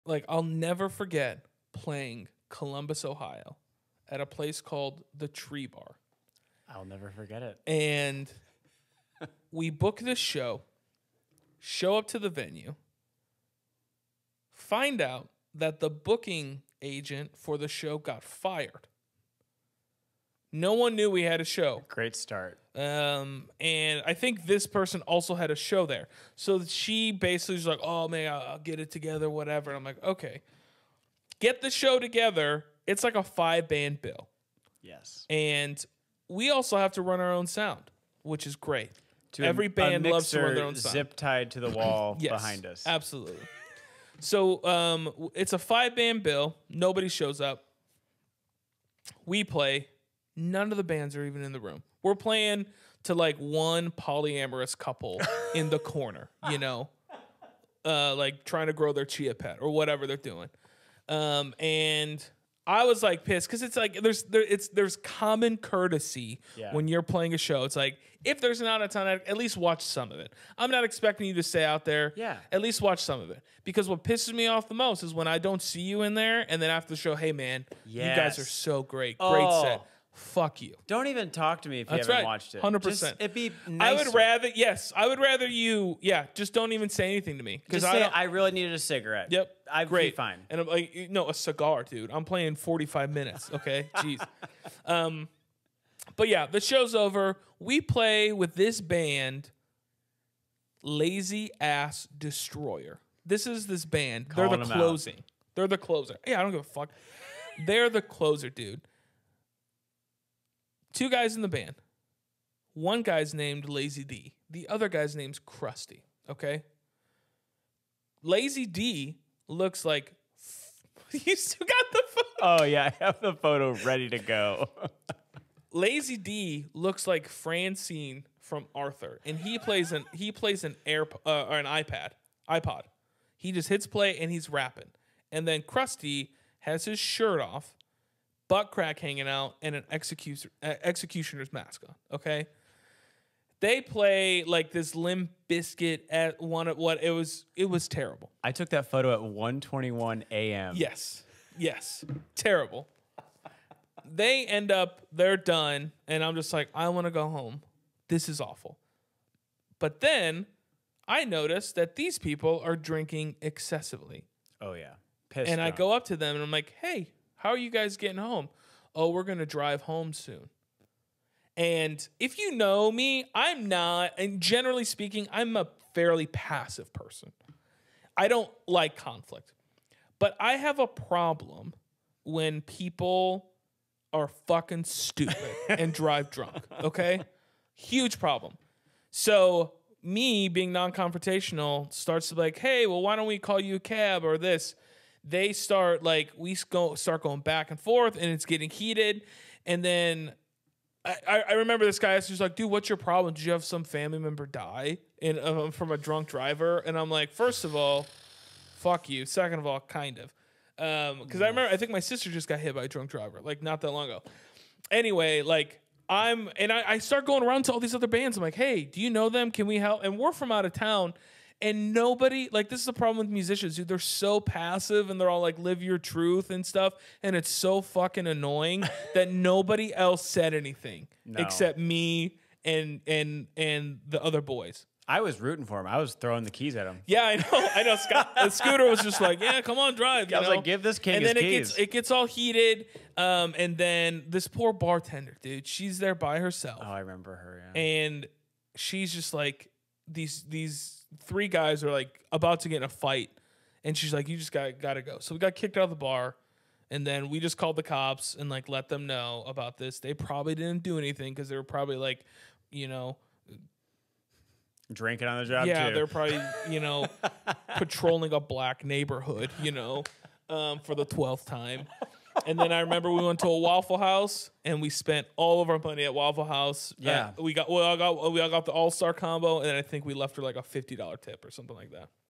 My Horror Story from Touring with my Band
Welcome to Saint Aubin Central the podcast of musician, Saint Aubin. Join him each week as he irreverently rants about music and culture.

In this clip, Saint Aubin recaps one of his most memorable stories from being on the road.